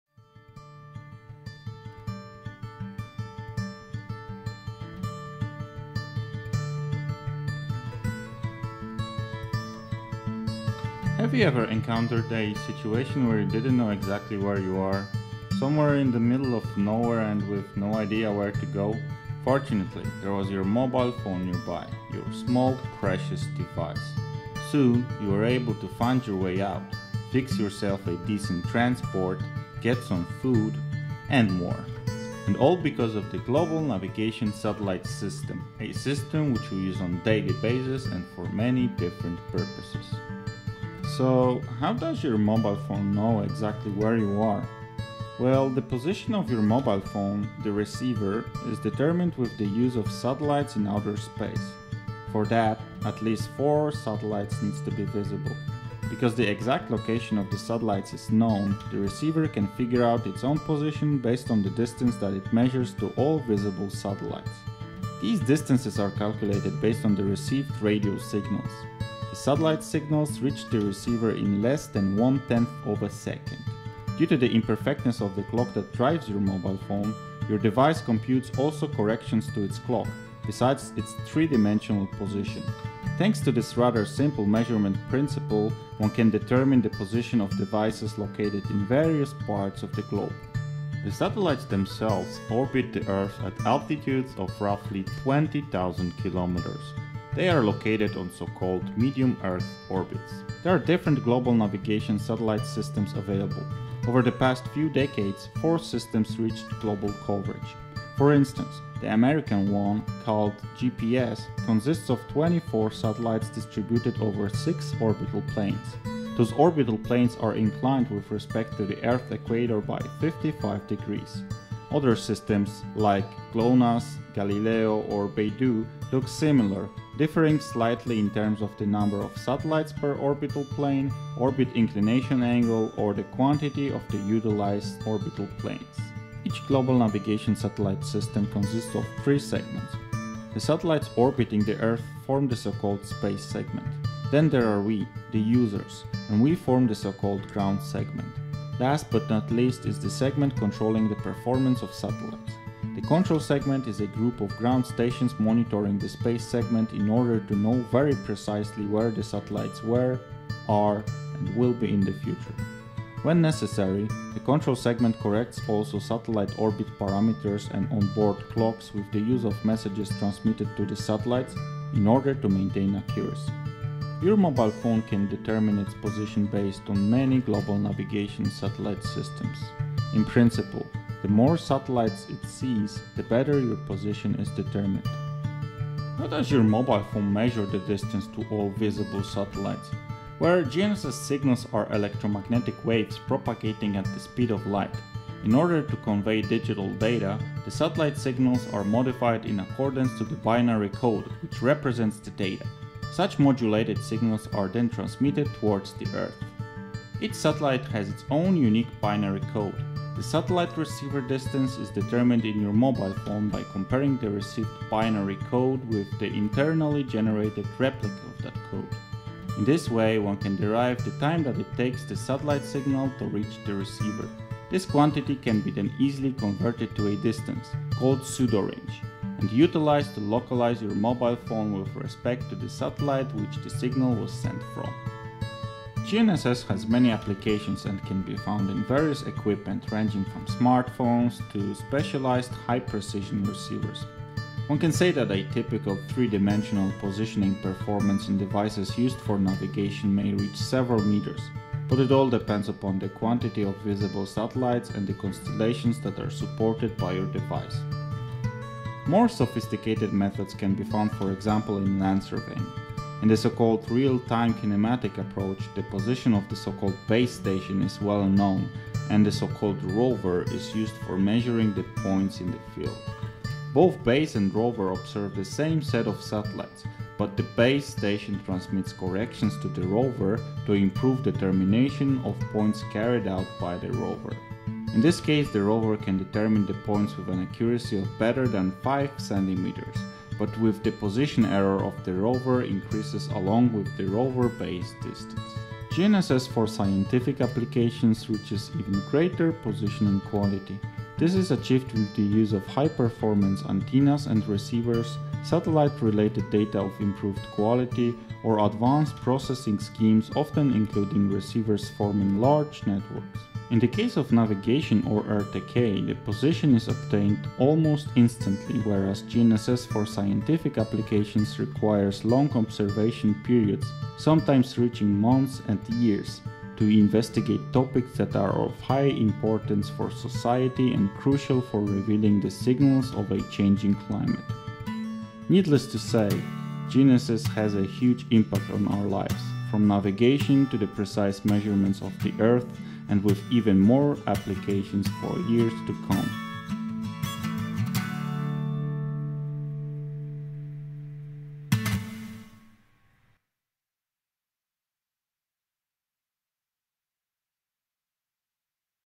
Navigation in your hand
How does your mobile phone know exactly where you are ? How can you navigate yourself with the use of a standard smartphone ? This is possible thanks to the Global Navigation Satellite System, a system which we use on daily basis and for many different purposes, from navigation to precise measurements of the Earth.

Credits:
Script & Animation: Grzegorz Klopotek
Voice-over & Music: Grzegorz Klopotek
Editing & Images: Grzegorz Klopotek

Made with the support of Onsala Space Observatory, Department of Space, Earth and Environment, Chalmers University of Technology, Sweden.